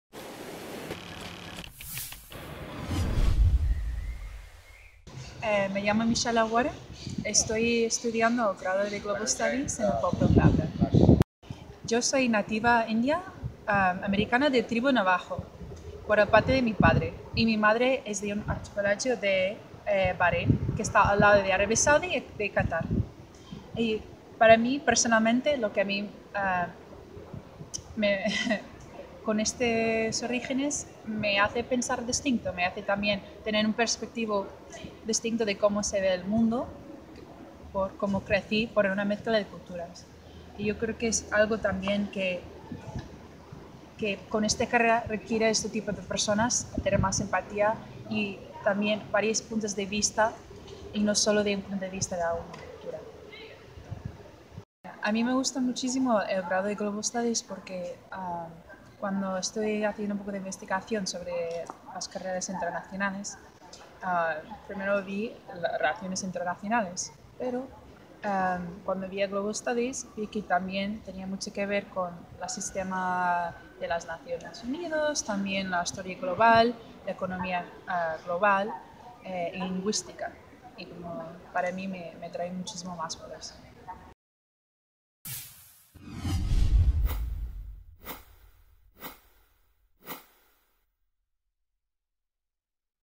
Entrevistamos a Michella Warren, estudiante del grado en Global Studies
Michella Warren acaba de empezar el nuevo grado en Global Studies de la UPF, una apuesta innovadora de la Facultad de Humanidades que pretende atraer estudiantes de todo el mundo. Ella es un caso singular. Nació en Pensacola (Florida) de padre nativo americano, de la tribu de los navajo, y de madre inmigrante de Bahrein.